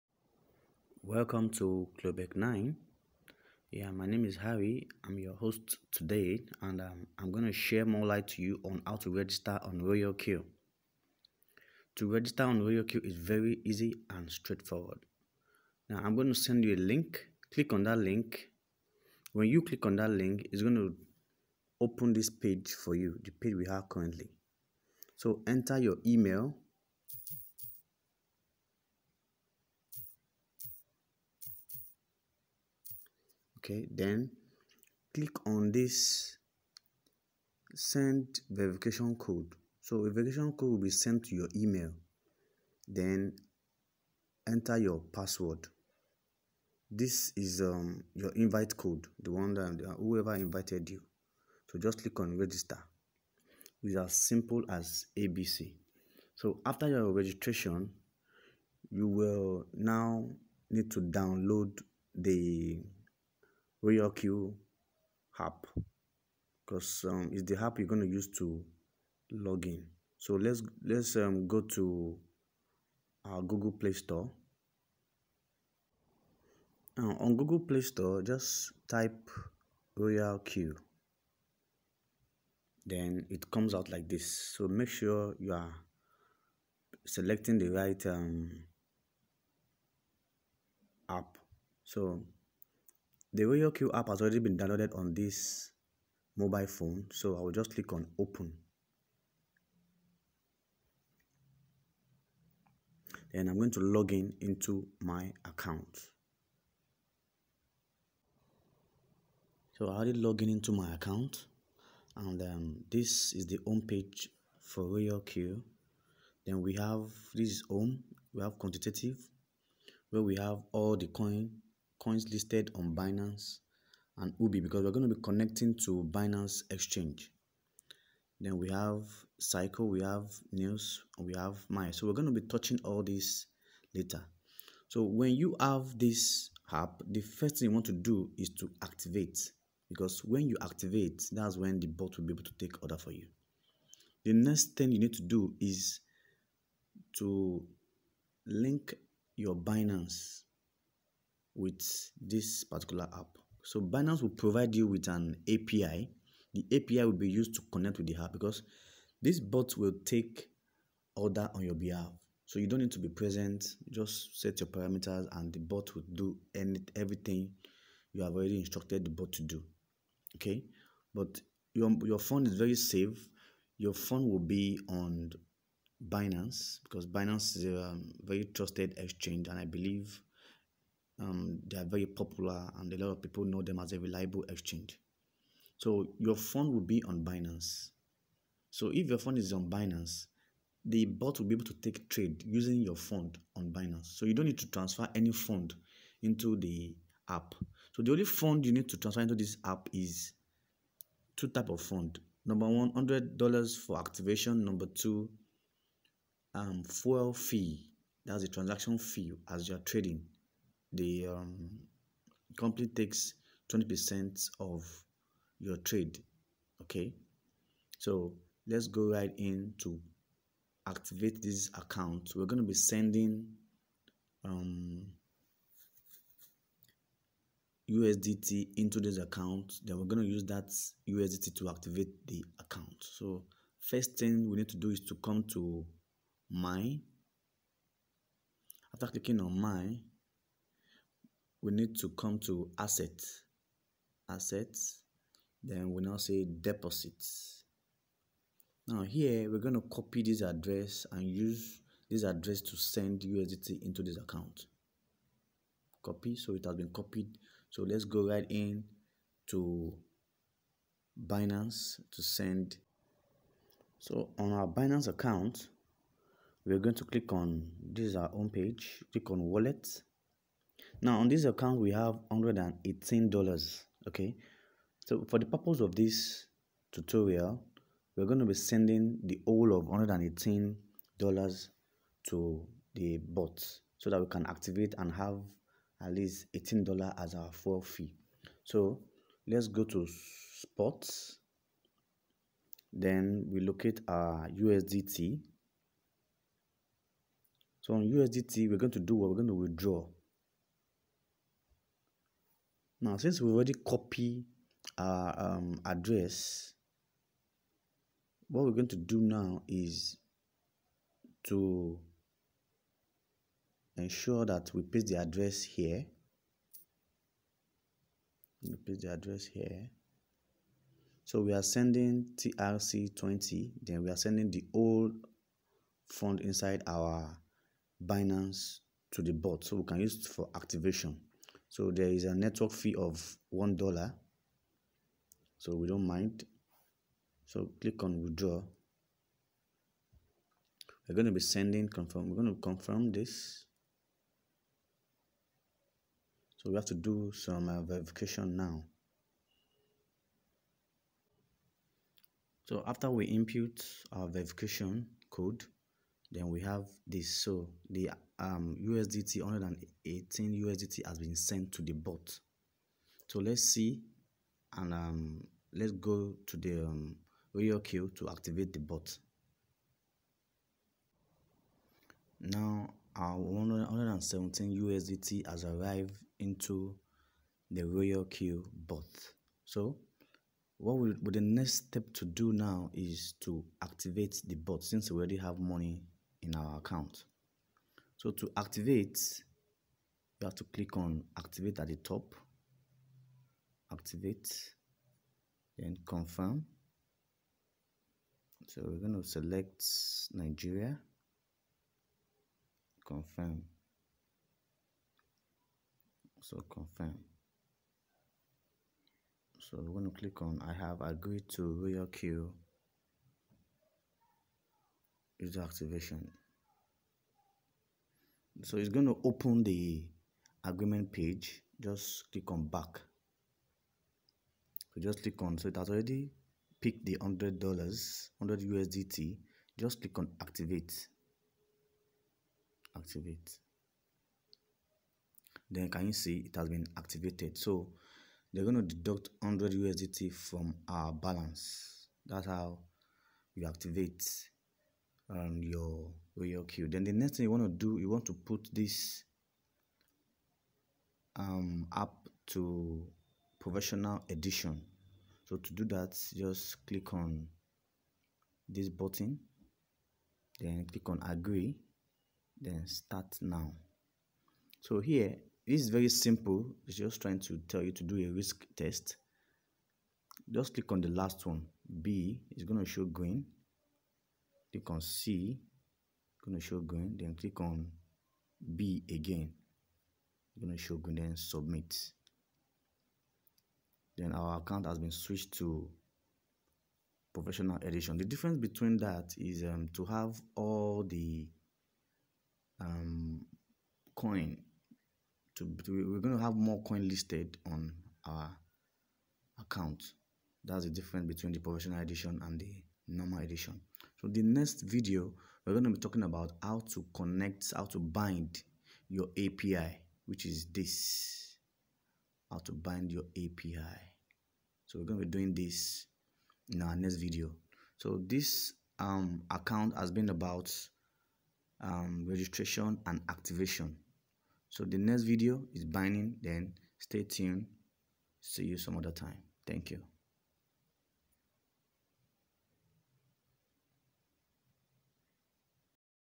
How to register and activate your Royal Q account.
Join the telegram for presentation
Technical training @11am Nigerian Time

Watch this videos you will understand how to start trading.